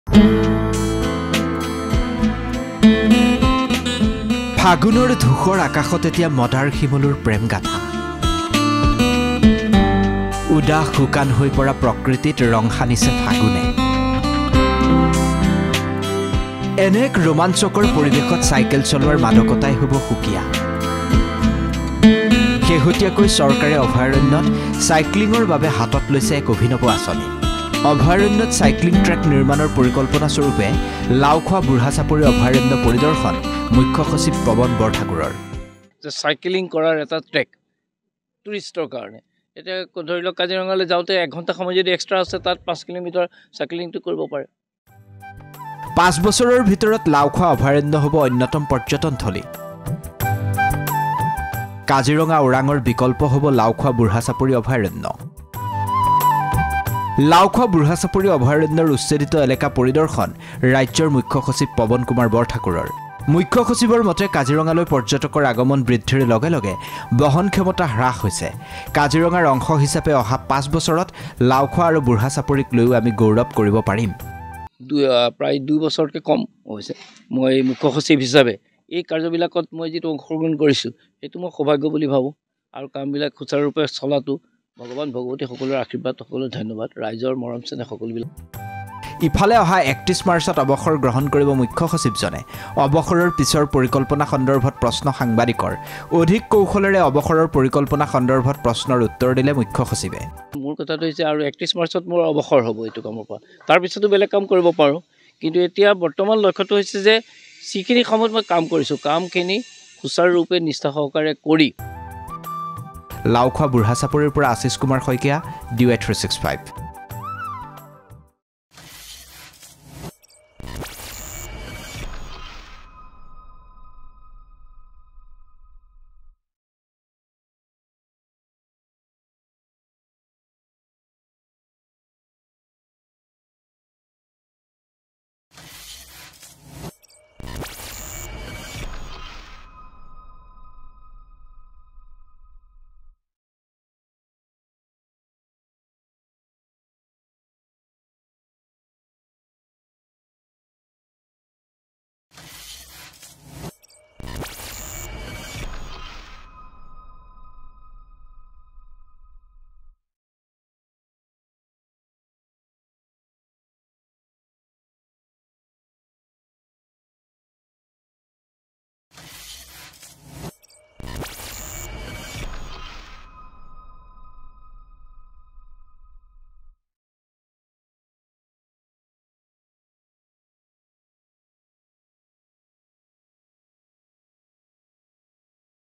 উতলা ফাগুনে ৰং সানিছে অৰণ্যত
উতলা ফাগুনে ৰং সানিছে অৰণ্যত। পলাশ, শিমলুৰ ৰঙচোৱা দলিচাৰ মাজেৰে এক ৰোমাঞ্চকৰ চাইক্লিং ট্ৰেক। লাওখোৱা বুঢ়াচাপৰিত চাইক্লিঙৰ এক অভিলাসী প্ৰকল্প হাতত লৈছে চৰকাৰে। চাওক আমাৰ এক প্ৰতিবেদন।